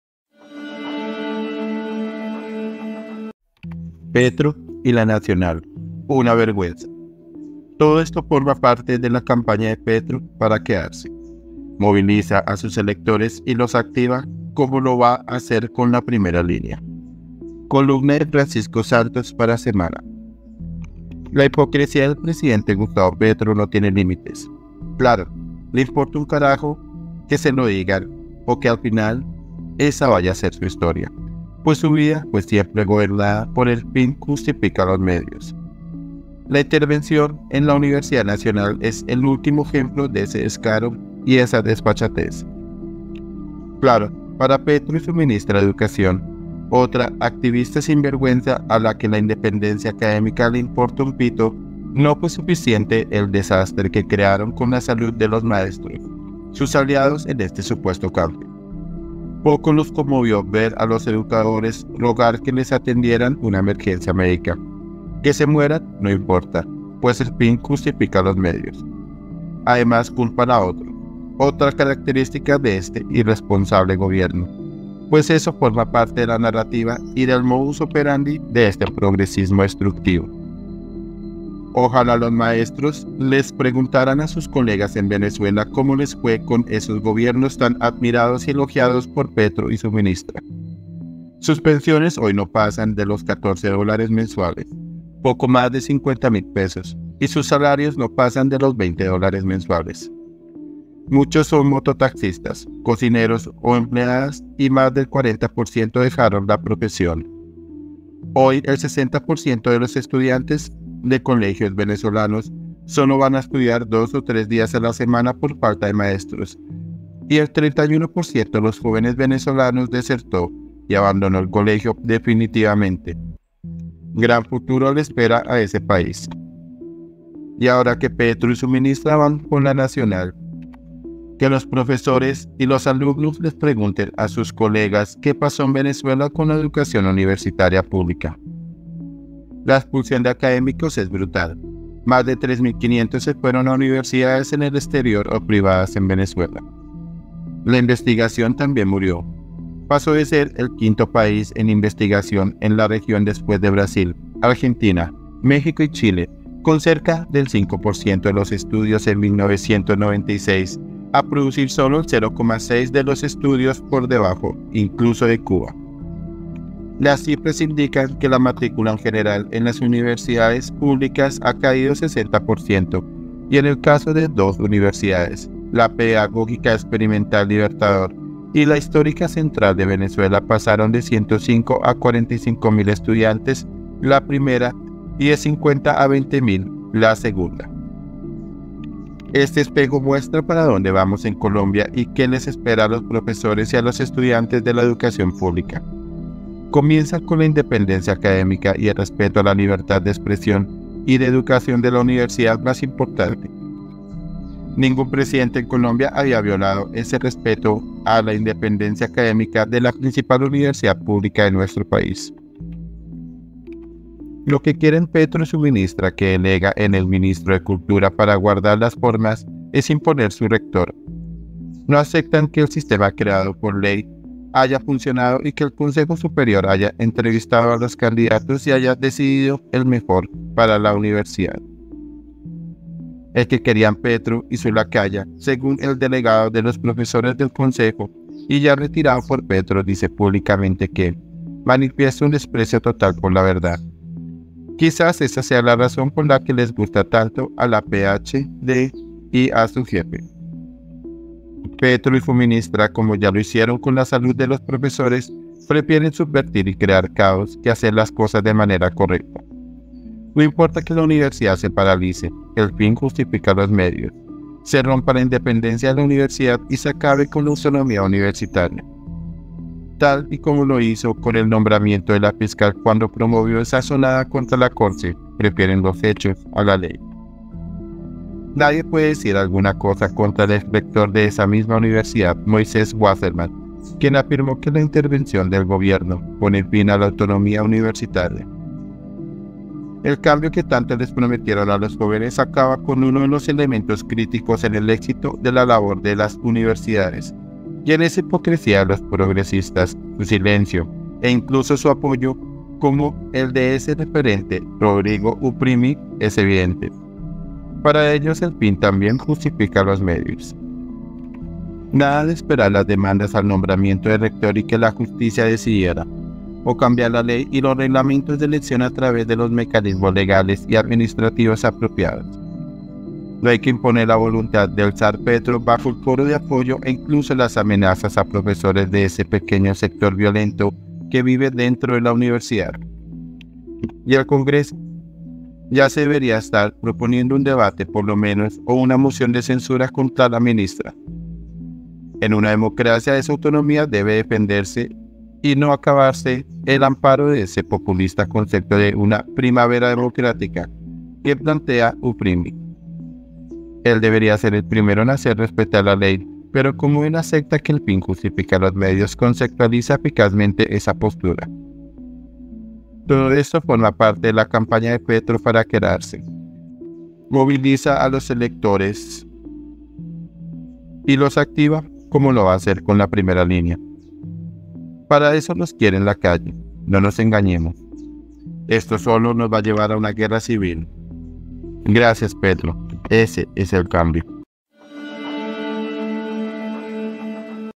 PETRO Y LA NACIONAL, UNA VERGÜENZA ⛔ Columna Francisco Santos
Todo esto forma parte de la campaña de Petro para quedarse. Moviliza a sus electores y los activa como lo va a hacer con la primera línea.
columna FRANCISCO SANTOS para SEMANA